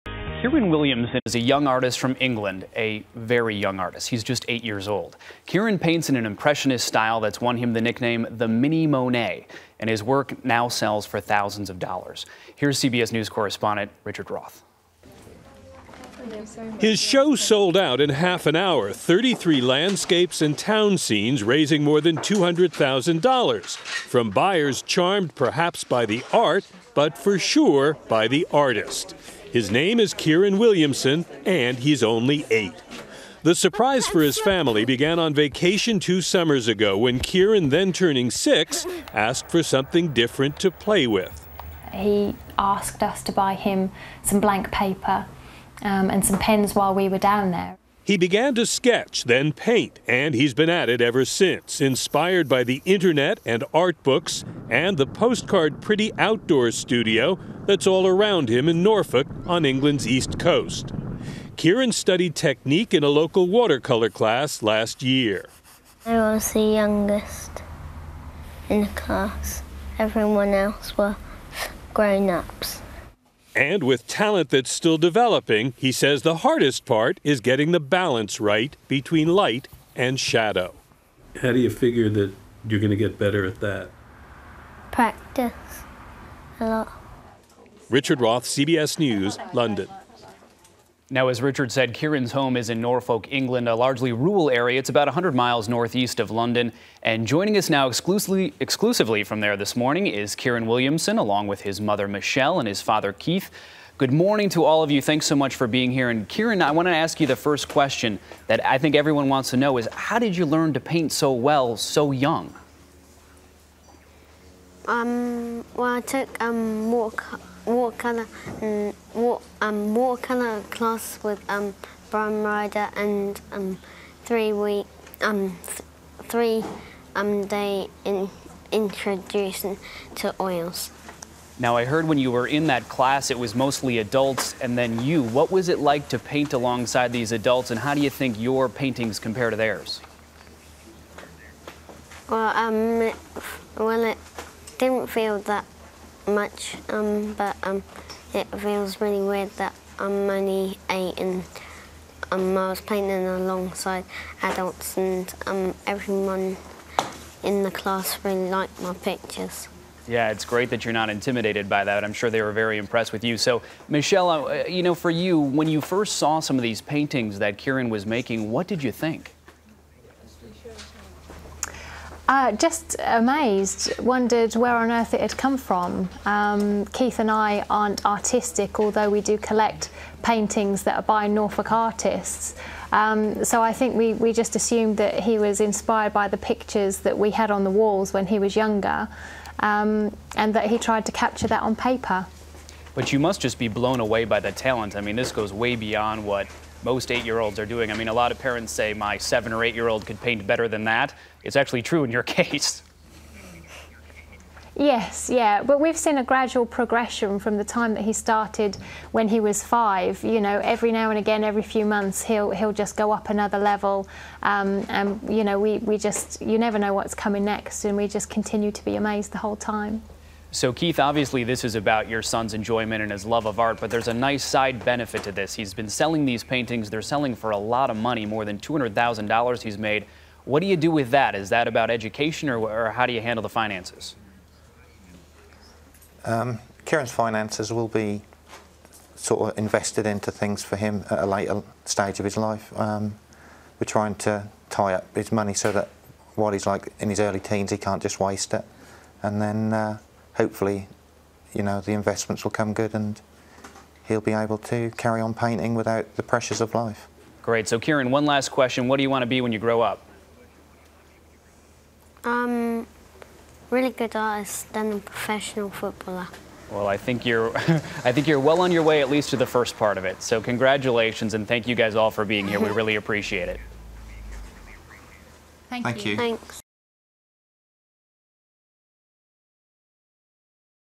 Kid Painter is Mini Monet
Richard Roth profiles 8-year-old painter Kieron Williamson, who paints like a master and earns thousands of dollars. Then, Ben Tracy interviews Kieron and his parents.